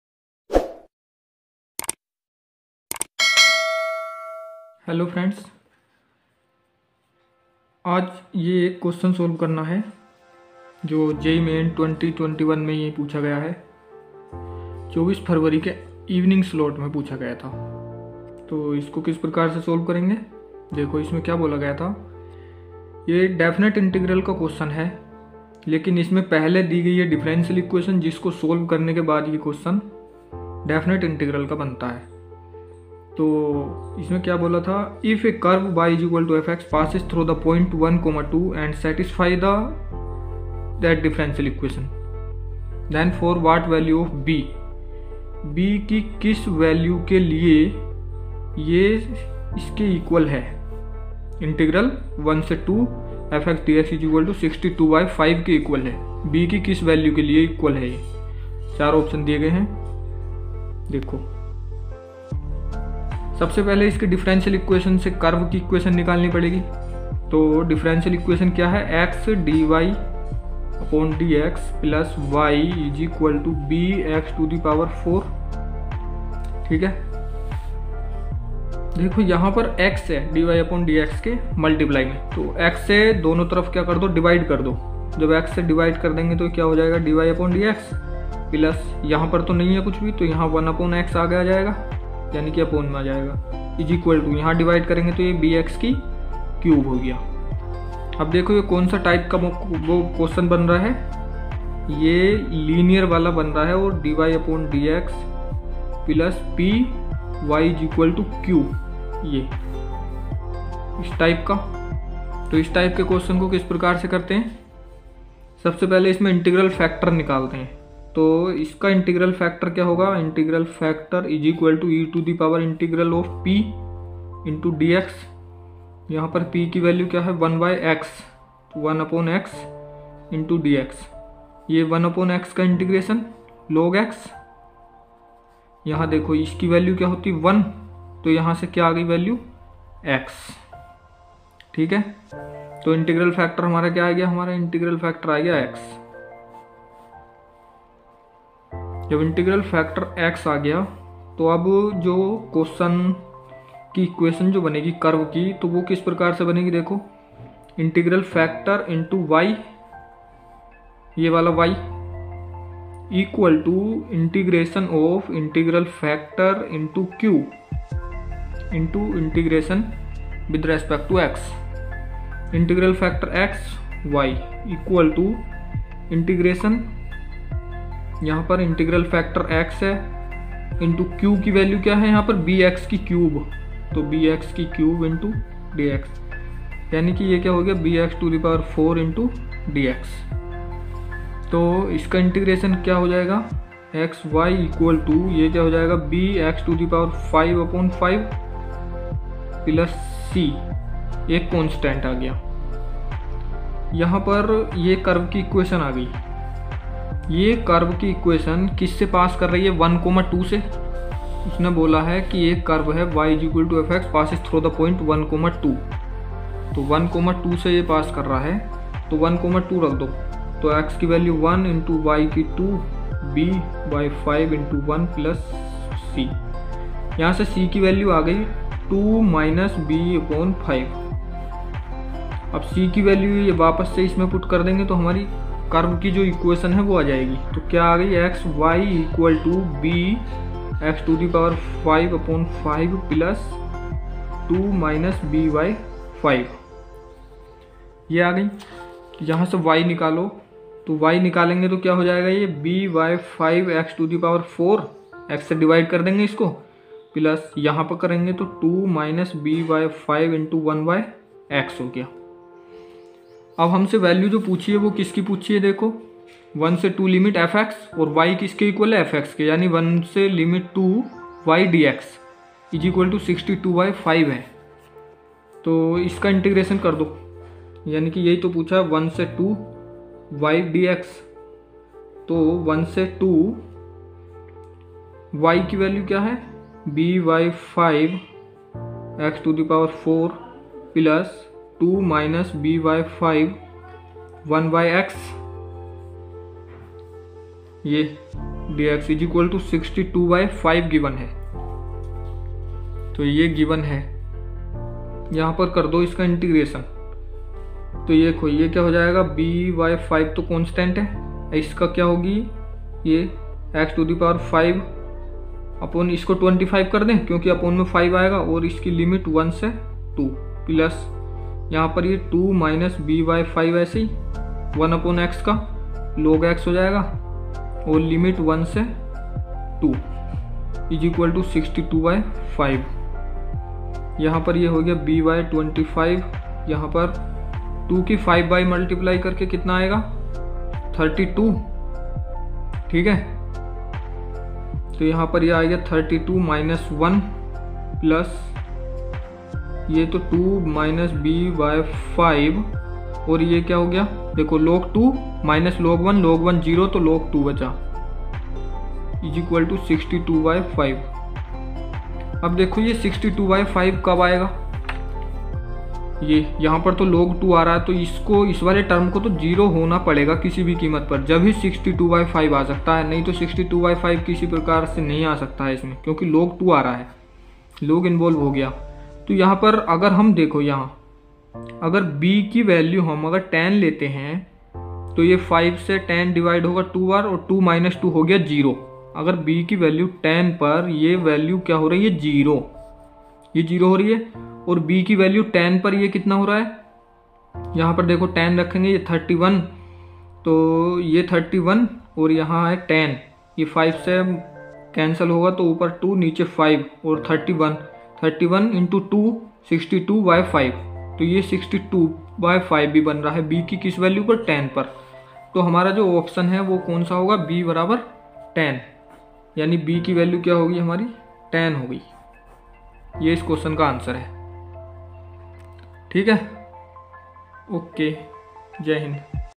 JEE MAIN 2021 (24th FEB) Question of definite Integral || Solution of definite Integral Question
Jee main 2021 problem of definite Integral.

today's video,I have solved a very important question of definite Integral of JEE MAIN 2021 (24th February).